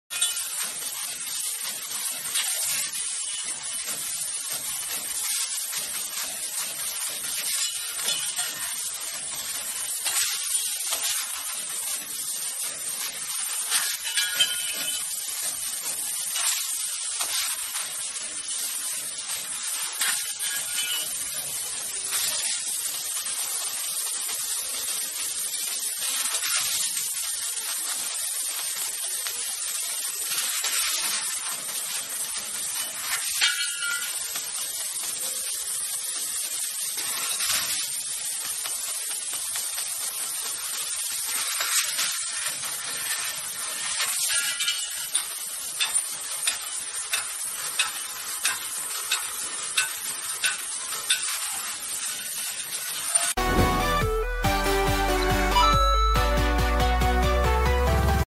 [SteviS Laser] High-speed stainless steel square tubing laser cutting machine with precision chuck
High-speed stainless steel square tubing laser cutting machine with high precision chuck.

We are metal sheets, plates, pipes and tubes laser cutting machines and laser welding machines manufacturer in China.